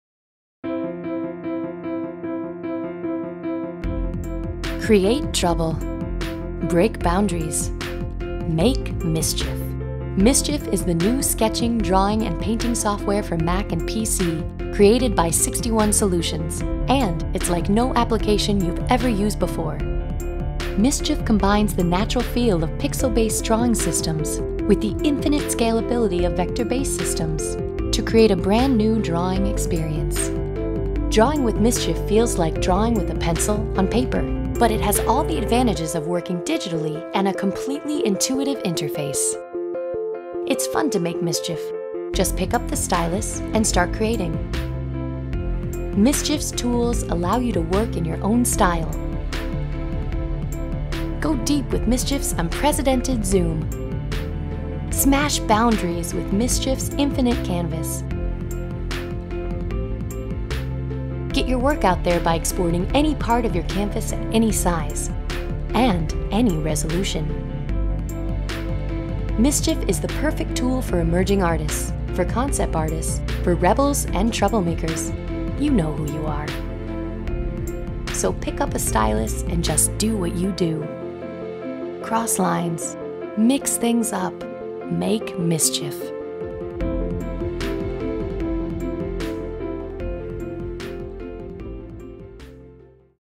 Mischief
Originalnye drawing program, thus you will have the feeling that you are working in raster graphics, but the data will be stored in a vector format. Many advantages in this, for example, you can zoom to infinity, the output file size will be small. Interface as simple as possible, nothing more in it, it is likely in the near future the program announced by a much louder.

More on:
GENTterritory.com